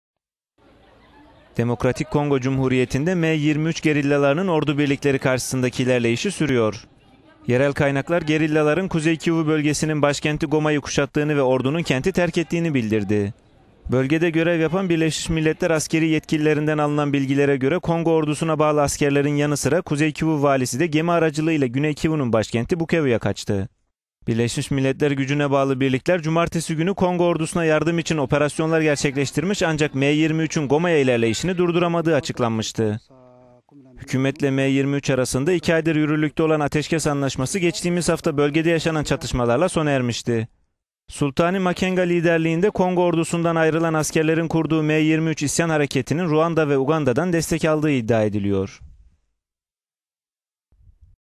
M23 gerillaları Goma'yı kuşattı
Demokratik Kongo Cumhuriyeti'nde M23 gerillalarının ordu birlikleri karşısındaki ilerleyişi sürüyor.

Yerel kaynaklar, gerillaların Kuzey Kivu bölgesinin başkenti Goma'yı kuşattığını ve ordunun kenti terkettiğini bildirdi.

Bölgede görev yapan BM askeri yetkililerinden alınan bilgilere göre Kongo ordusuna bağlı askerlerin yanı sıra Kuzey Kivu valisi de gemi aracılığı ile Güney Kivu'nun başkenti Bukevu'ya kaçtı.

BM gücüne bağlı birlikler cumartesi günü Kongo ordusuna yardım için operasyonlar gerçekleştirmiş ancak M23'ün Goma'ya ilerleyişini durduramadığı açıklanmıştı.

Hükümetle M23 arasında iki aydır yürürlükte olan ateşkes anlaşması geçtiğimiz hafta bölgede yaşanan çatışmalarla sona ermişti.

Sultani Makenga liderliğinde, Kongo ordusundan ayrılan askerlerin kurduğu M23 isyan hareketinin Ruanda ve Uganda'dan destek aldığı iddia ediliyor.